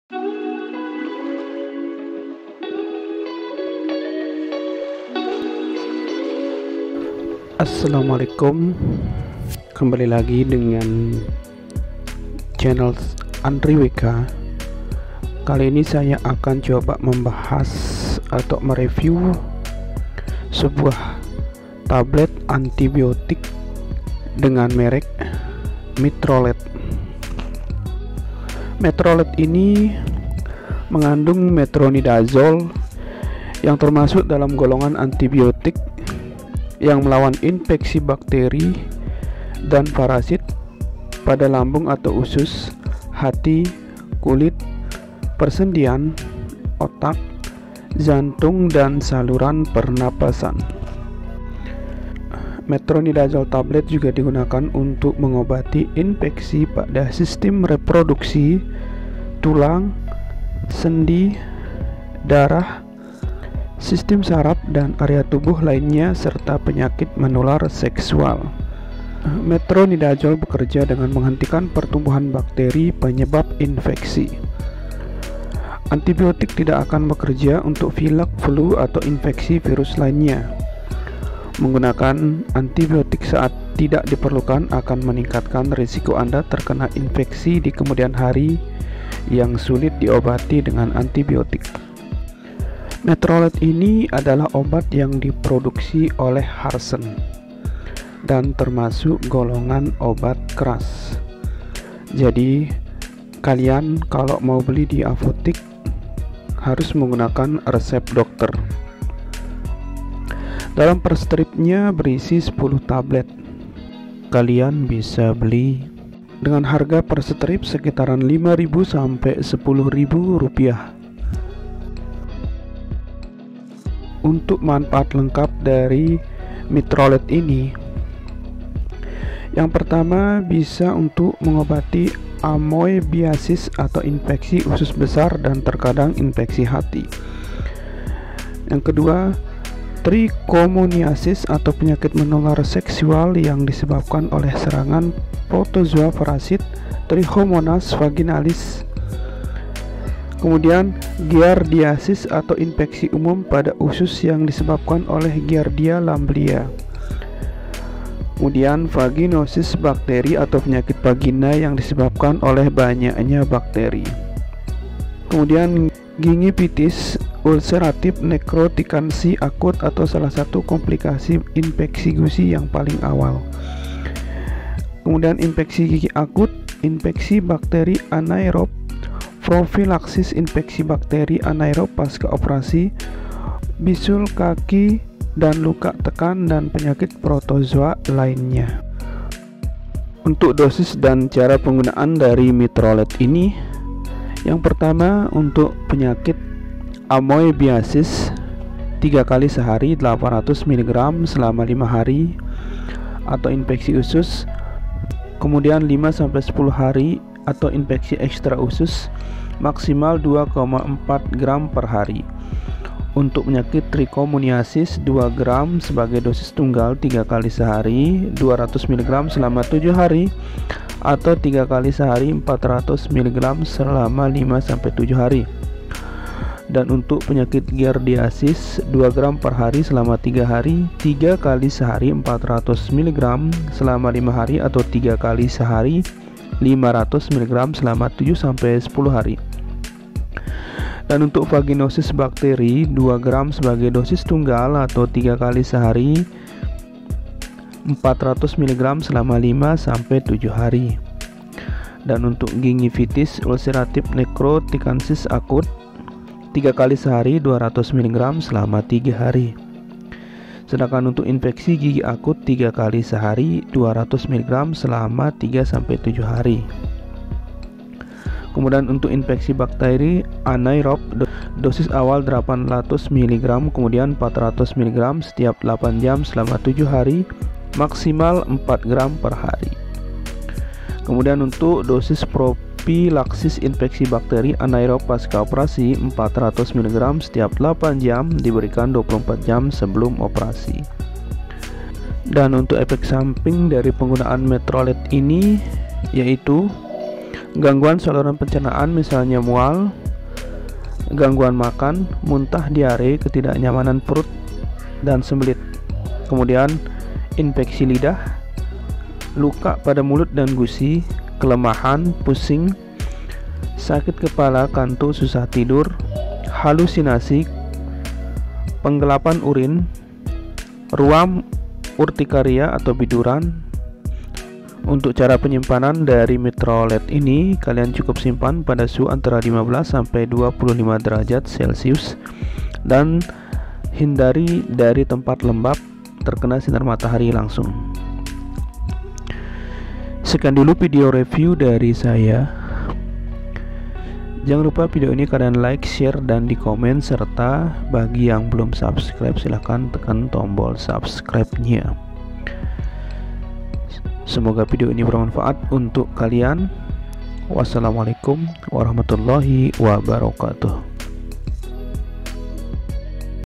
Review Metronidazole Untuk Keputihan dan Infeksi Bakteri serta Parasit | Metrolet
Metrolet mengandung metronidazole 500 mg yang dapat digunakan untuk mengobati infeksi bakteri pada vagina, lambung, hati, kulit, persendian, otak, dan saluran pernapasan.